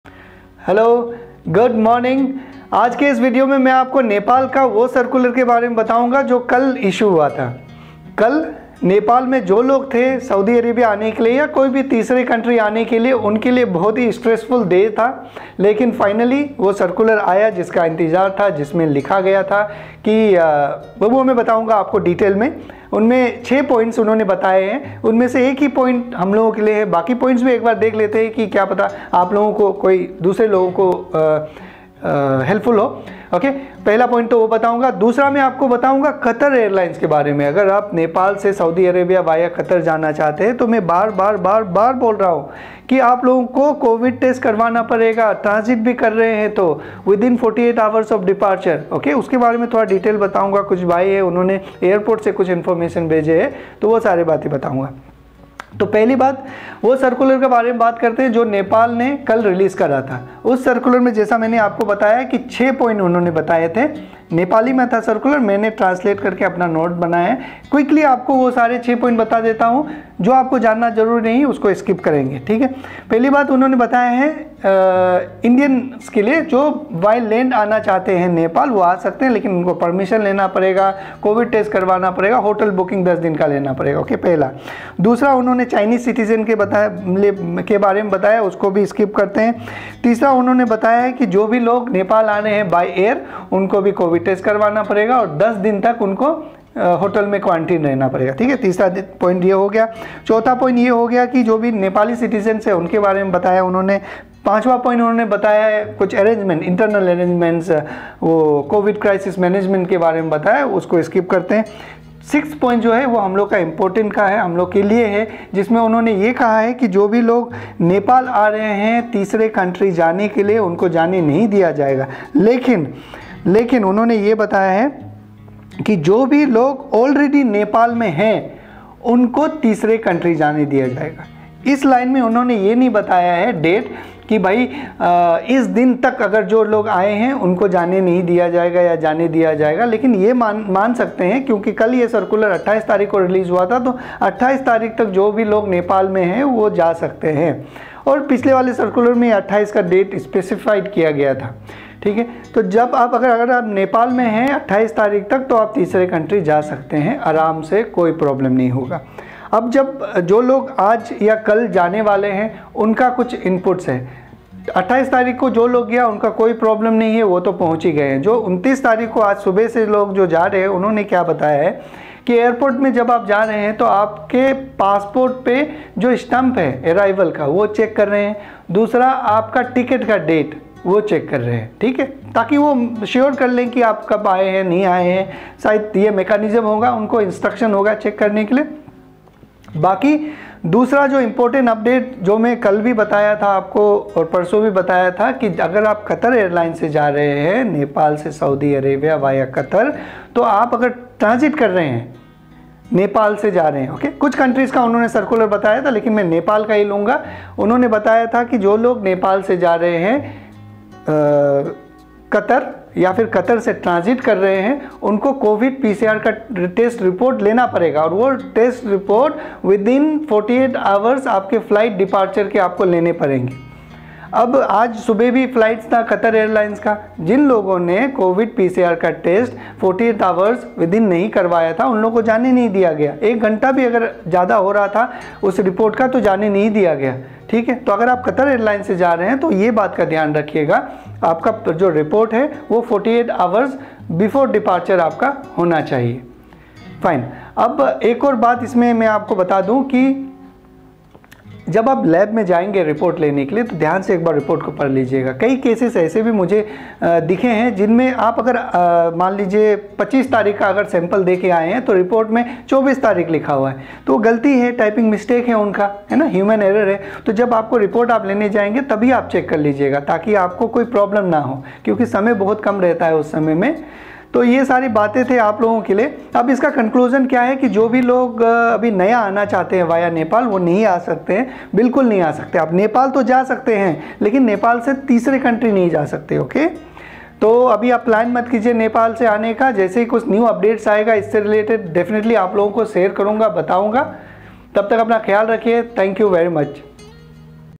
Nepal Travel update 29th April 2021, Qatar Transit MUST have COVID report within 48 Hours
rjworld2020 // Video # 162 // 29th April 2021
Those who are in Nepal, no need to worry, you can travel to 3rd Country.
Those who are traveling by Qatar airlines, Must have a COVID PCR test report. Test must be done within 48 hours of departure